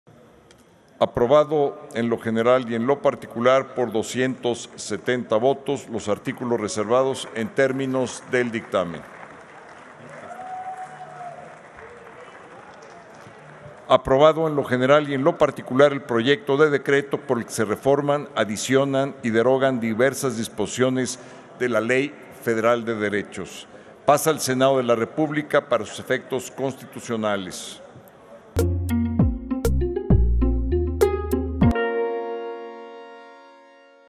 Votación en lo general / Ley Federal de Derechos
Con 270 votos a favor, 217 en contra y 1 abstención queda aprobado, en lo general y en lo particular, el dictamen que reforma, adiciona y deroga diversas disposiciones de la Ley Federal de Derechos. Pasa al Senado.